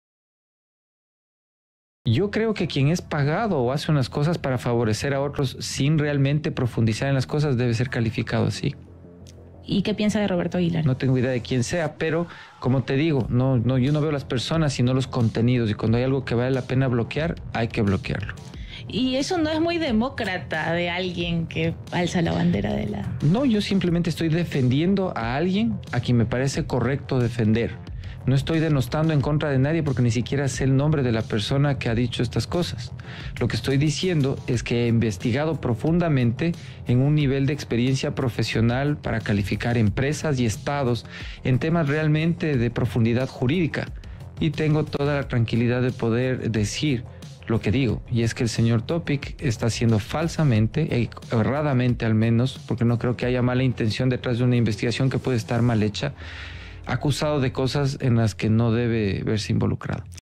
¿Usted cree que Roberto Aguilar es un mercenario de la prensa?
Mira las declaraciones de Pedro Freile